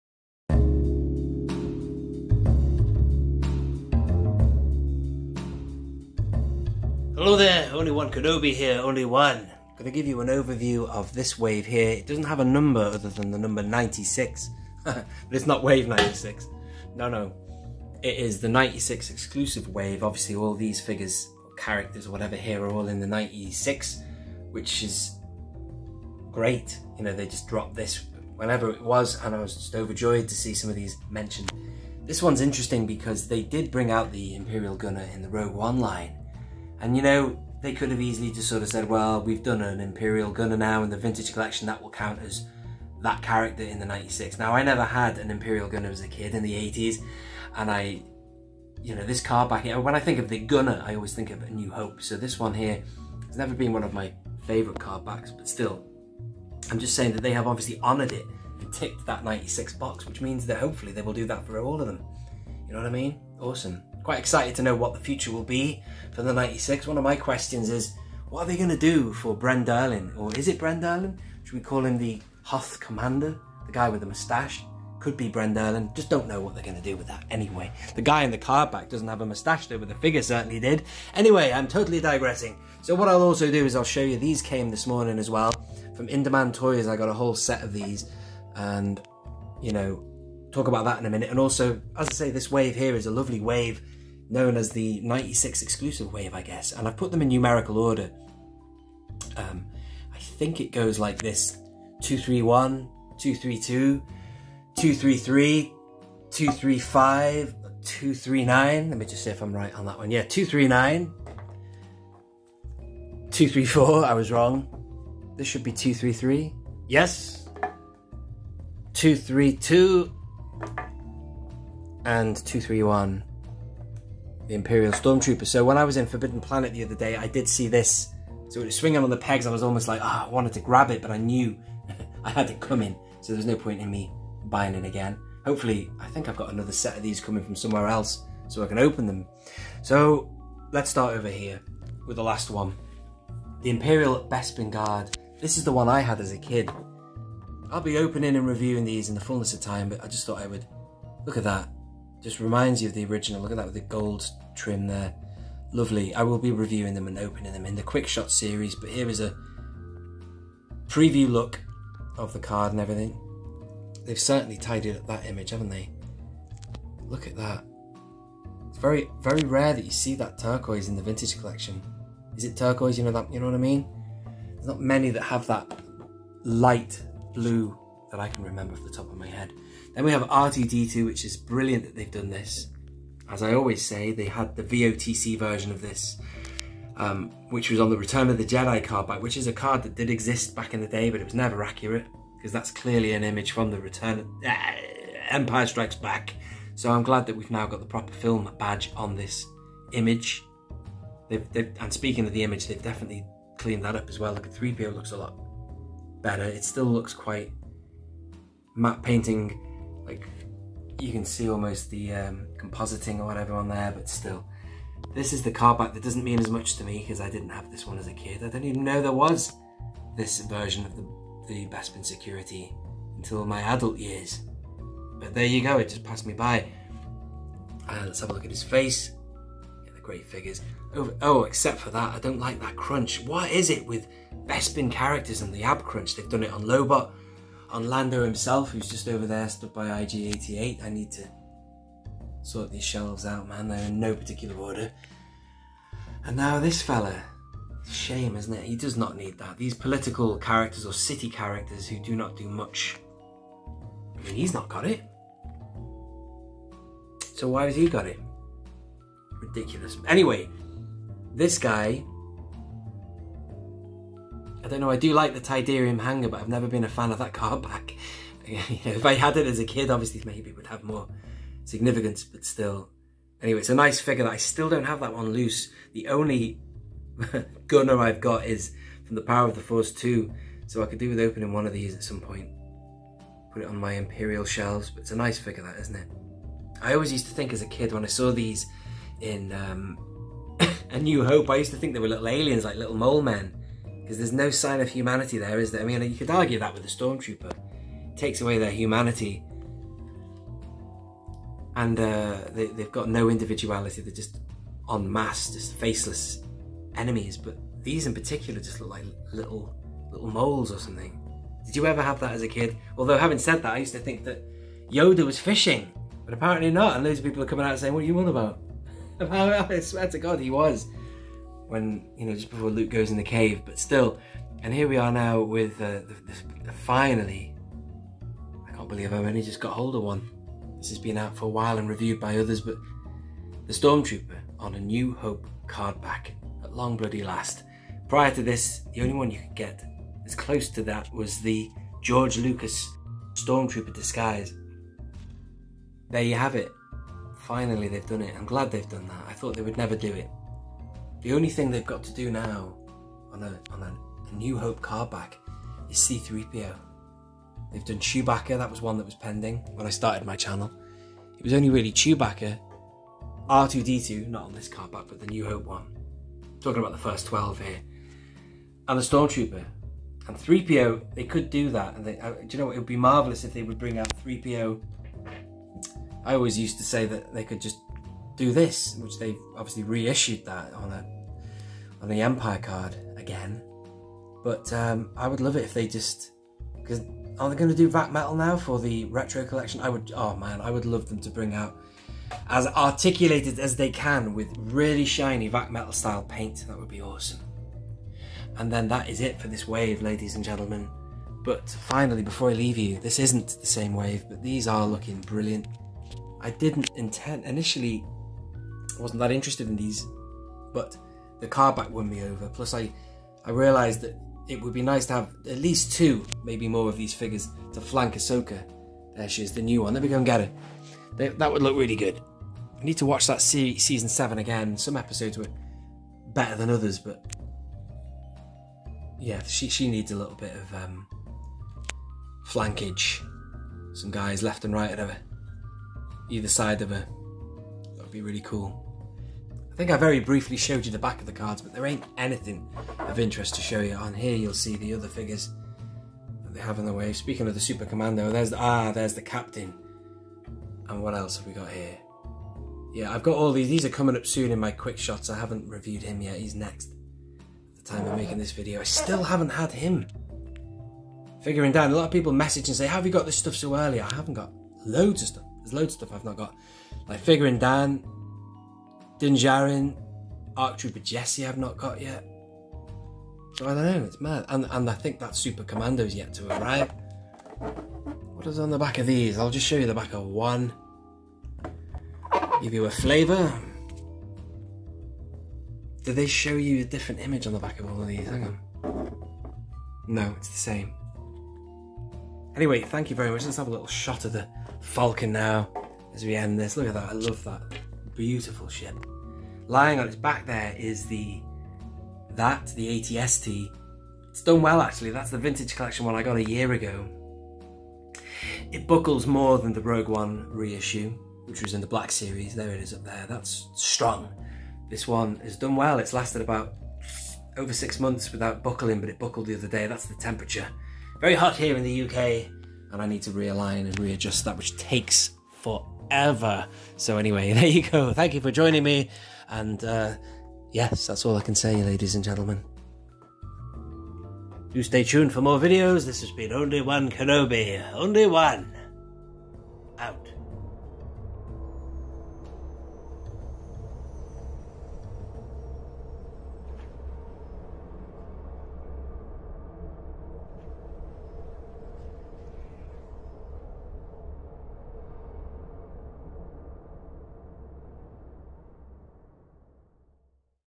The 96 EXCLUSIVE Wave | Star Wars 3.75 Vintage Collection 2022
Nice to see a chunk of 'the 96' fulfilment in the Star Wars Vintage Collection! Bring on more @hasbropulse 🙂 O.O.K 👌🏼💙

Only One Kenobi
Wirral,
CH31 9GE
UK

Twitter / Facebook / Instagram @onlyonekenobi

THANK YOU PATRONS & CHANNEL MEMBERS 😘